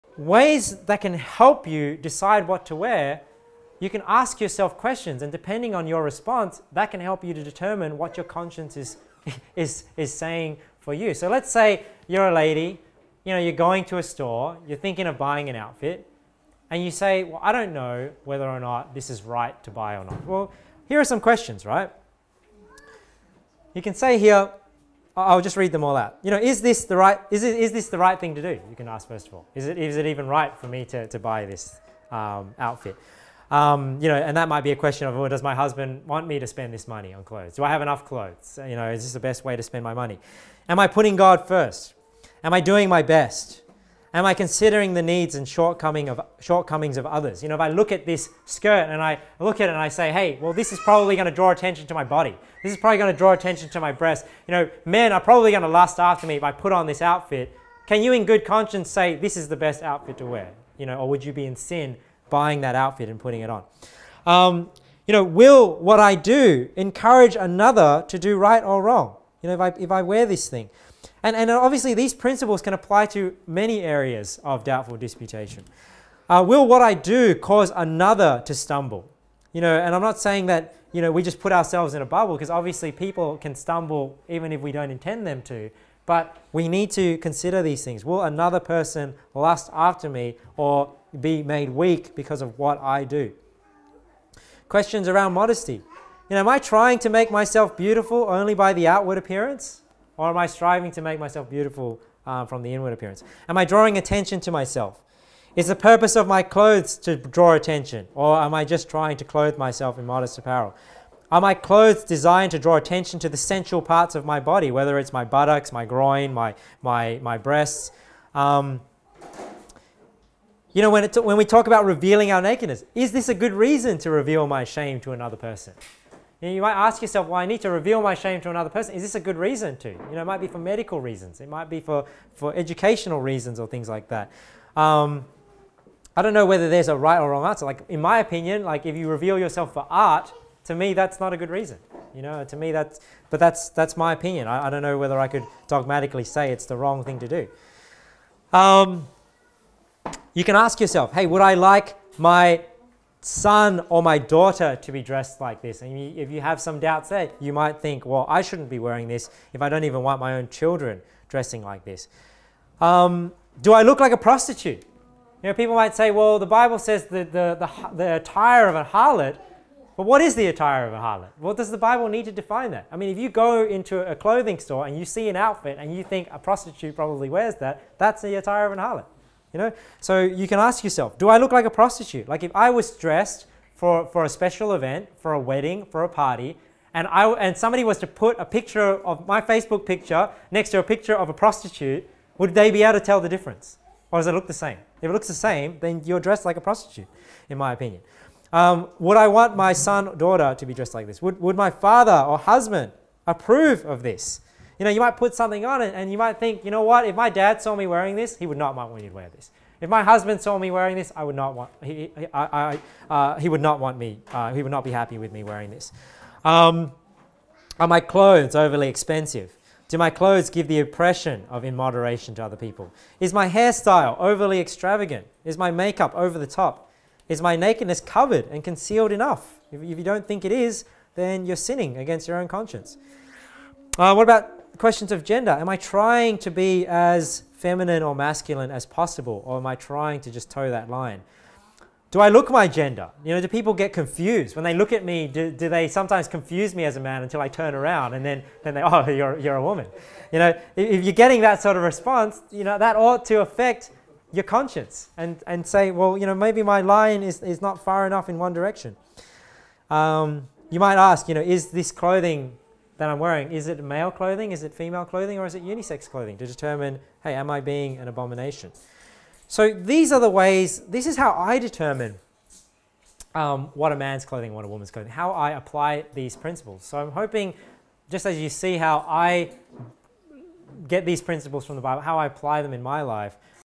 What Clothes Should I Wear? | Victor Tey | 01/May/2016
This is a sermon preached by Victor Tey at The Church in Punchbowl on Sunday, 1st May 2016.